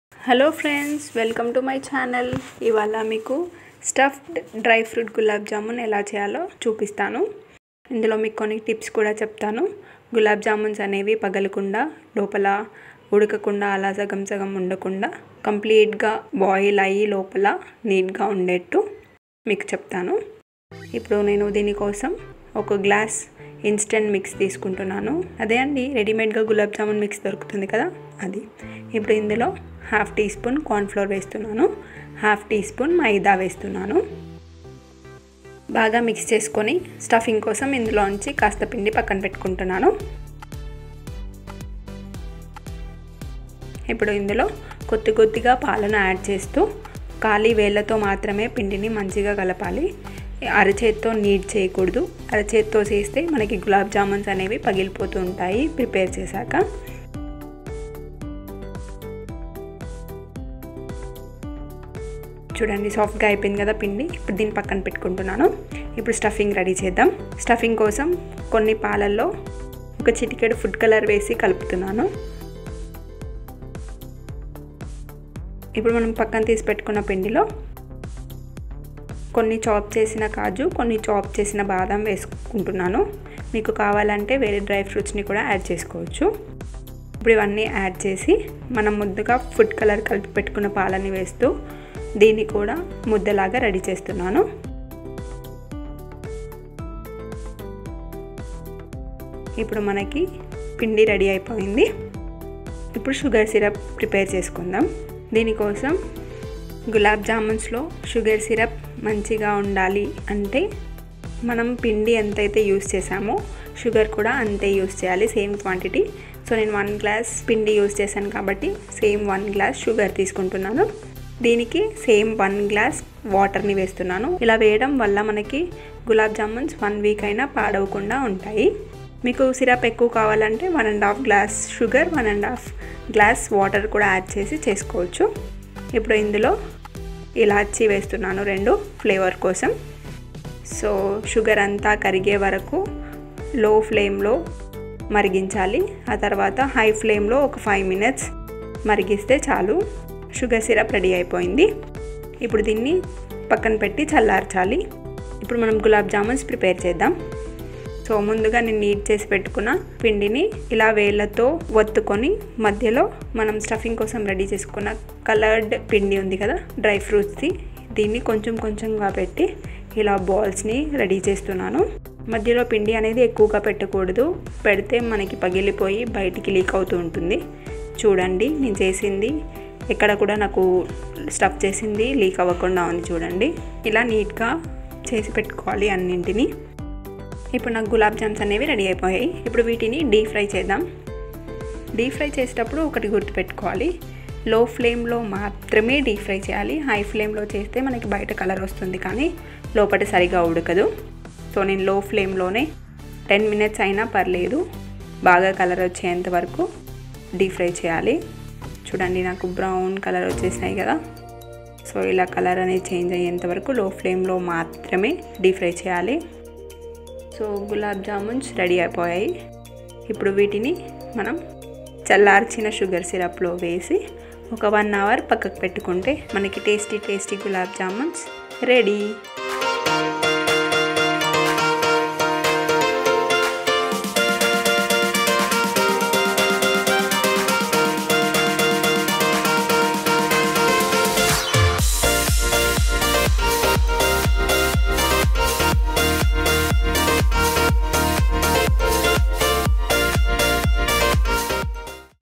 గులాబ్ జామూన్ పగలకుండ simple tricks తో ఒక్కసారి ఇలా ట్రై చేయండి..చాలా soft గా వస్తాయి
DRY FRUIT GULAB JAM